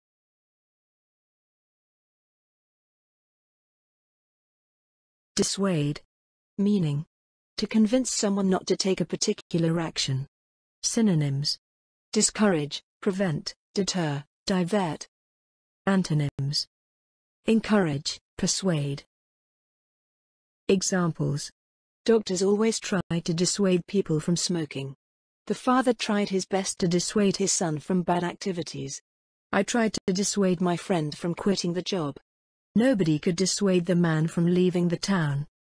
What Does Dissuade Mean? [Dissuade Meaning] | Learn English Words With Examples
Dissuade Meaning: to convince someone not to take a particular action
Synonyms: discourage, prevent, deter, divert, etc.
Antonyms: encourage, persuade, etc.

For career guidance and opportunities send your CV on